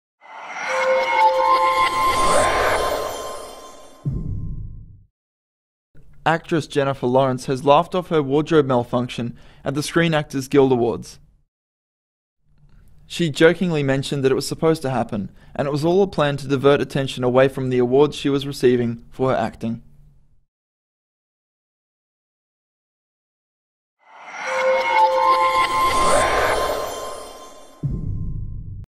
Jennifer Lawrence laughs off her wardrobe malfunction
The actress has joked about her dress disaster at the Screen Actors Guild Awards.
0:29